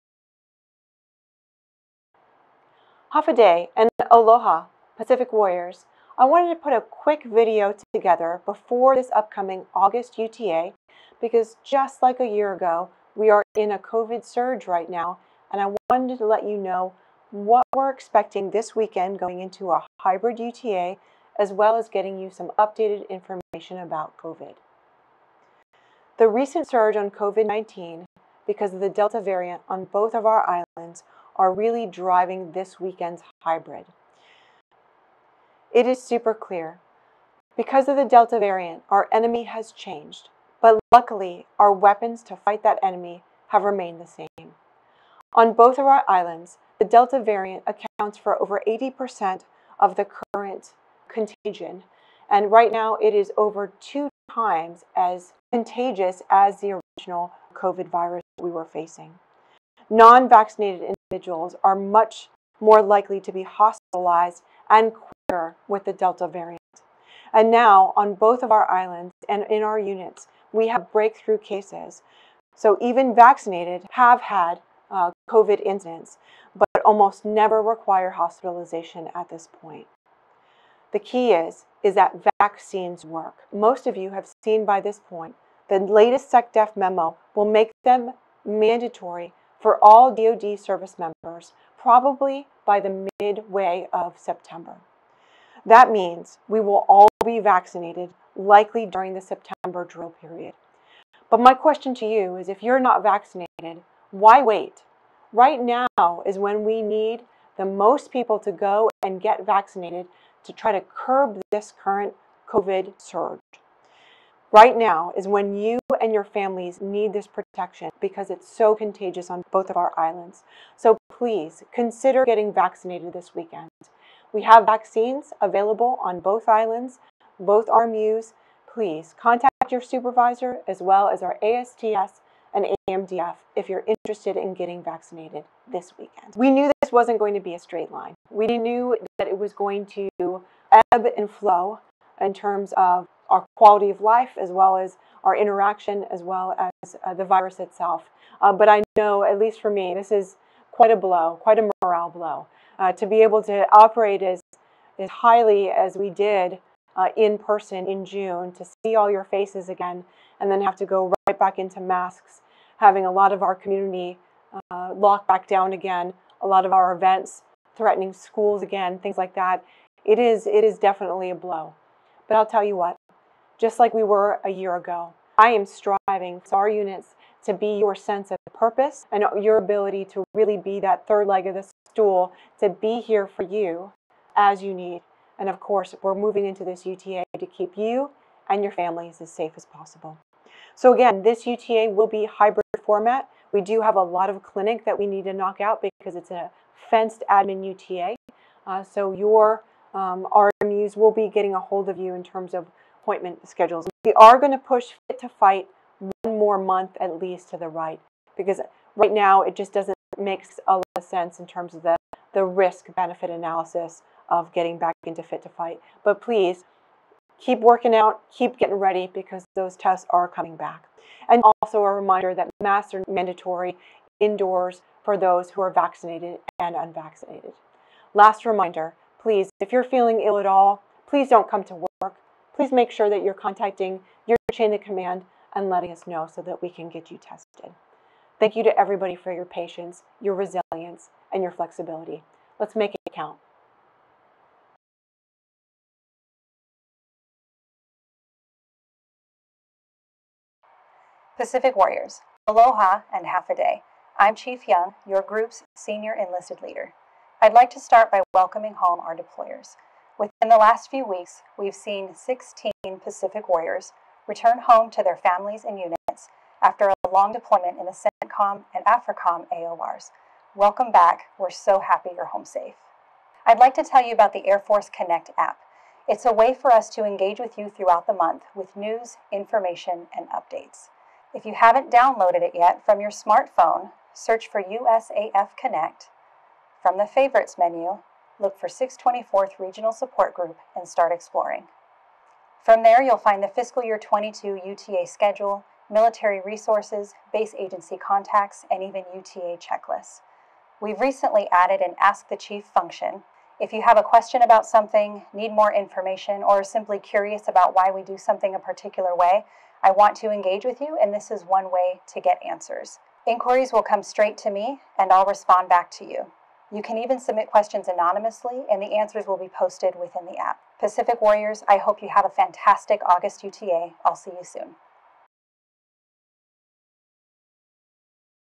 624th Regional Support Group August Pre-UTA message
624th Regional Support Group August Pre-UTA message
JOINT BASE PEARL HARBOR-HICKAM, HI, UNITED STATES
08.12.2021

Pacific Warriors,

Welcome back to August UTA, and I hope you all had a safe and relaxing July off! As you all know, COVID-19 is now surging on both of our islands, and accordingly, we will be going back to a hybrid drill. Luckily, it is our admin fenced UTA, and there are a lot of training and administrative items we can do virtually, but we will be running full medical clinics for all units. We have made the decision to delay Fit To Fight fitness testing one more month, to ensure the safety of you and your families.

Our fight against COVID is clear...our enemy has changed with the Delta variant, but our weapons stay the same...and our #1 weapon is the vaccine. Thank you to those who have already gotten vaccinated, we're above the Air Force average at 70% fully immunized in Guam, and 74% in Hawaii. As you've all heard, the SecDef will be directing vaccines no later than mid-September, so for those of you not vaccinated, plan on receiving the first vaccine during September UTA. We will ensure you are on status for both vaccines, at least an RMP, in case of any side effects. But why wait...now is when we are all *most at risk* with the community transmission rates of Delta variant, so please consider getting your vaccine this drill weekend. Please remember that masks are now mandatory again indoors, no matter your vaccination status. Finally, if you are feeling ill at all, please stay home and contact your chain of command.

Meanwhile, we have a lot to catch up on this weekend; we have Commander's calls, DEOCs debriefs, OPSEC and other training, and most of us will have to re-initial security clearances. We also have a lot of medical appointments, so please ensure you are on time to those. I am so proud of everything we accomplished during the June UTA...you all made incredible progress in our local readiness exercises, and really impressed all of our visitors from 4th Air Force and AFRC.

A last note about funding; the command was underfunded in Annual Tour funds for the first time I've ever seen. I'm truly appreciative to everyone who chose to waive their ATs...we ensured everyone has a good year, but if you still want to serve your Annual Tour, please work closely with your chain of command, as we are committed to fund every tour we are able. We are trying to lean forward now in our planning for next fiscal year so that we can ensure the best use of your time and our dollars to get us ready for the next deployment cycle.

I am so proud of all of you...your hard work, your flexibility and resilience, and your commitment to your Reserve family continues to inspire me!

Col Sia Shinas

Film Credits: Video by James Bowman
624th Regional Support Group

Air Force
624th Regional Support Group
Reserve Citizen Airmen
August Pre-UTA message